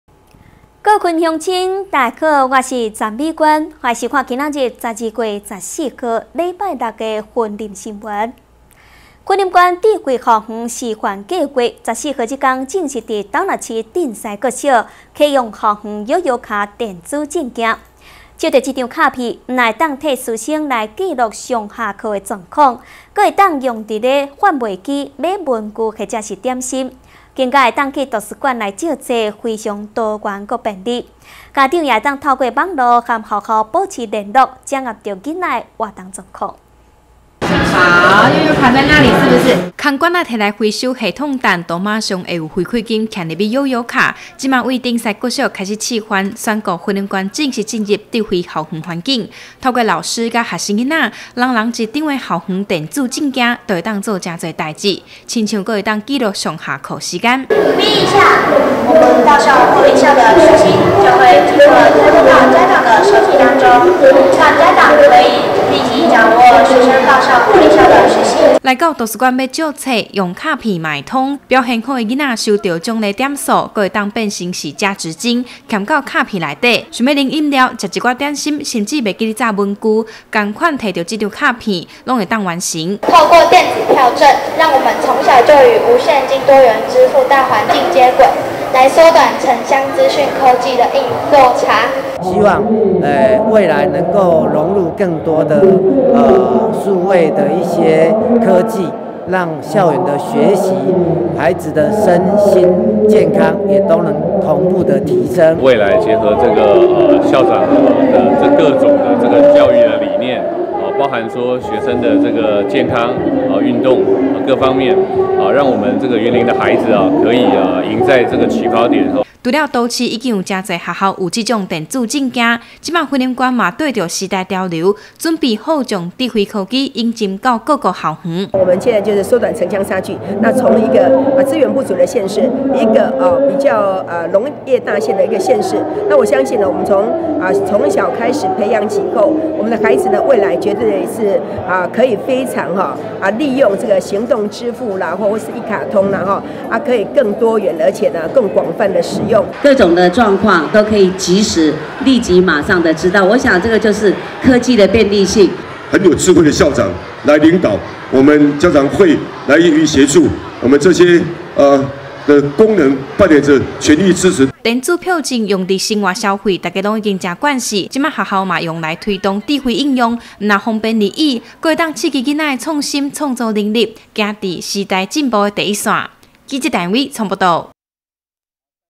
雲林新聞網-鎮西智慧校園開啟新紀元 縣長張麗善:逐步打造全縣智慧E校園
因應智慧科技時代來臨，雲林縣智慧校園示範計畫正式於斗六市鎮西國小啟用校園悠遊卡票證，未來更將逐步引進智能硬體至全縣各校園。〈以上新聞可從top NEWS APP收視〉